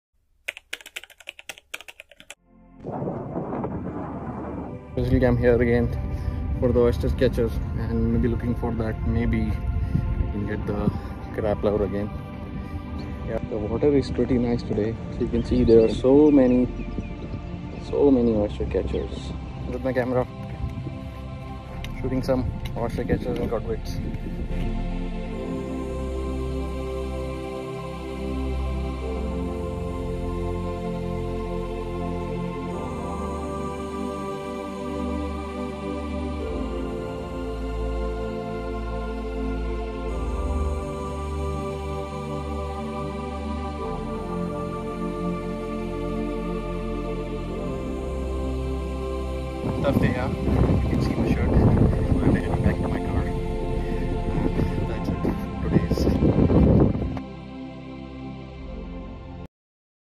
Oystercatchers,Godwits and waders at al khobar,Saudi arabia
while visiting my local patch at the sea side for birds photography i found some oystercatchers ,godwits,curlews,redshank and so many shore birds